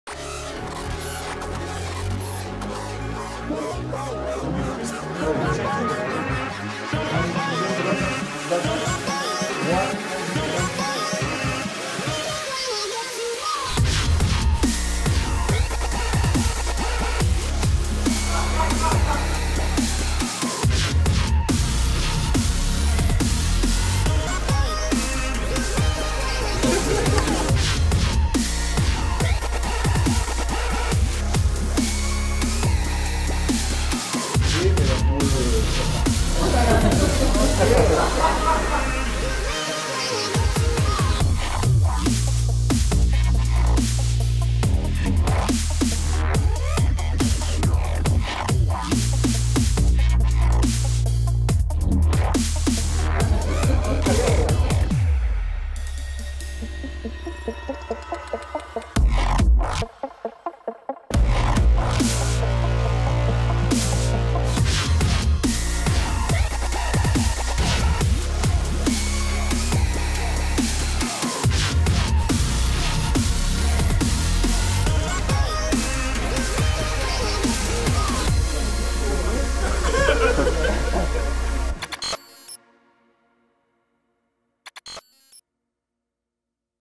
Maillot Third - 𝕄𝕒𝕜𝕚𝕟𝕘-𝕠𝕗 🎬
Plongez-vous au coeur du tournage photos & vidéos du MaillotThird avec nos Stadistes ! ✨